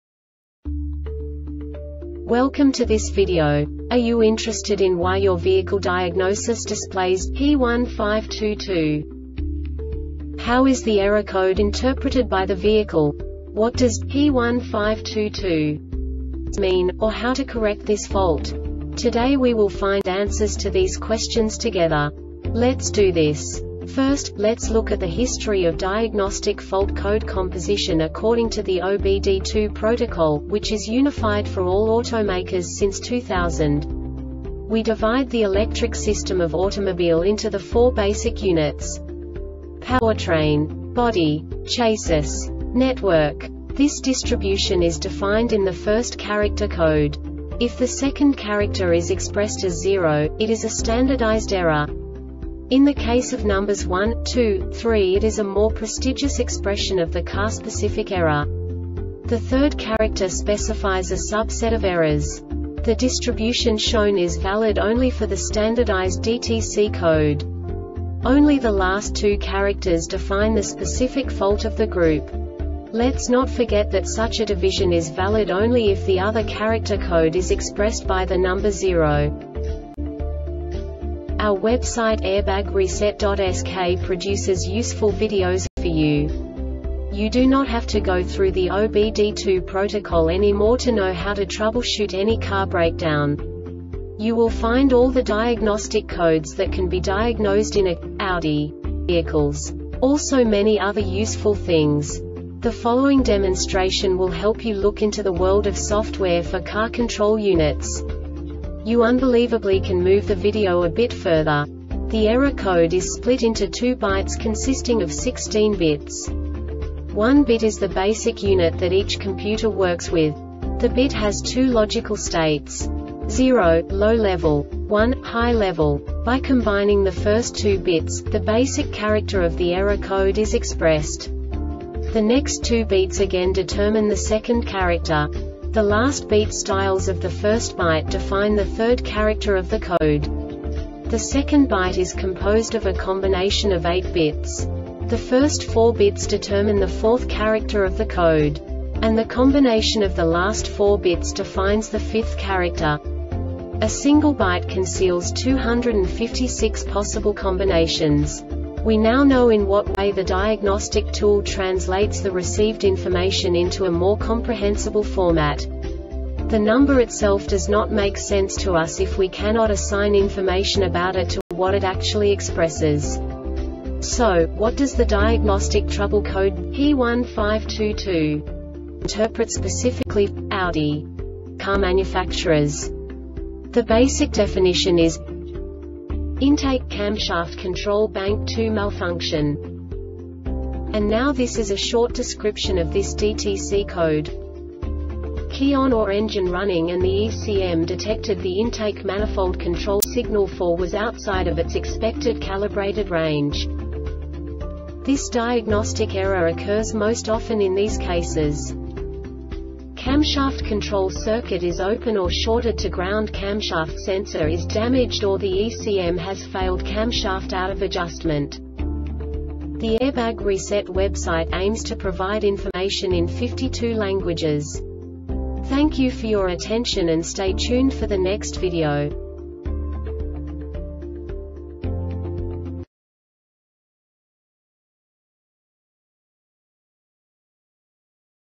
DTC Audi P1522 Short Explanation
Contents:
0:21 Basic DTC analysis according to OBD2 protocol standard.
1:48 Insight into programming
2:58 A diagnostically understandable description of the Diagnostic Trouble Code
3:05 Basic error definition

Make:
Audi
Code:
P1522
Definition:
Intake Camshaft Control (Bank 2) Malfunction
Description:
Key on or engine running; and the ECM detected the intake manifold control signal for was outside of its expected calibrated range.
Cause:
Camshaft control circuit is open or shorted to ground
Camshaft sensor is damaged or the ECM has failed
Camshaft out of adjustment